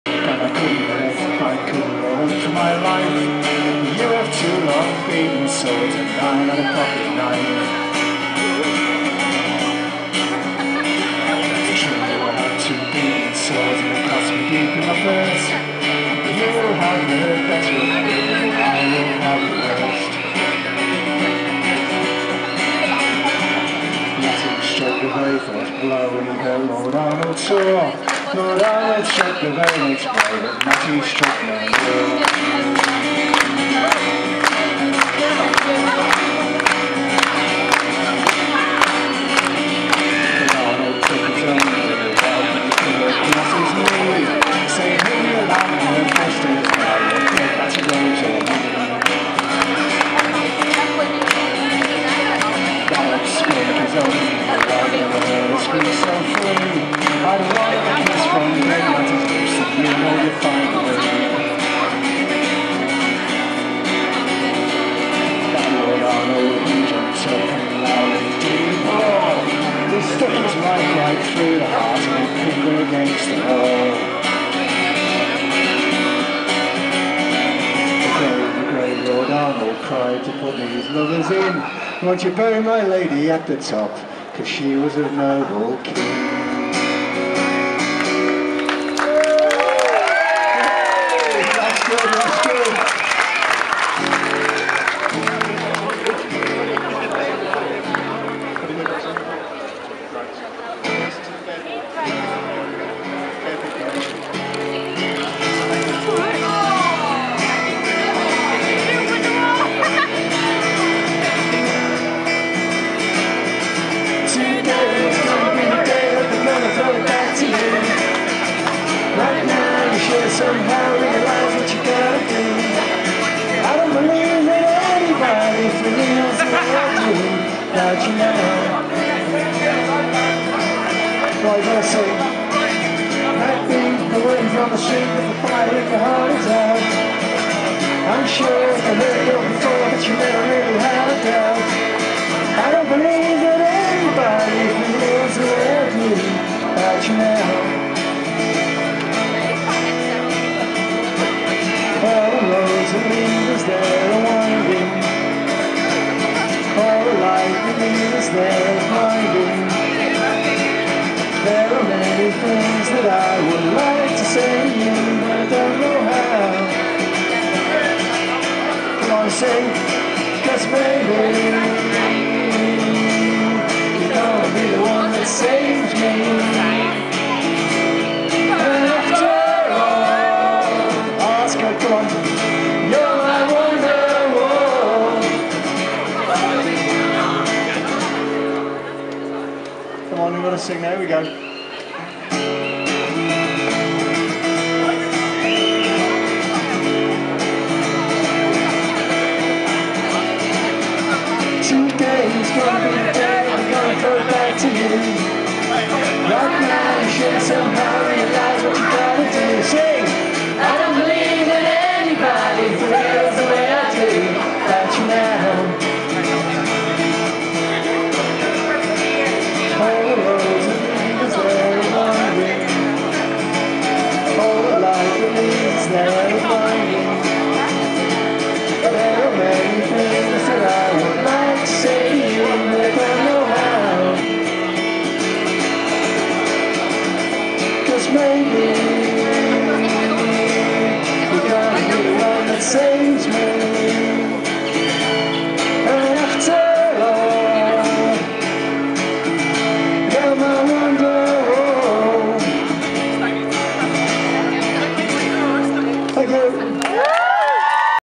Policeman Singing In Birmingham
Policeman singing with guitar for charity in Birmingham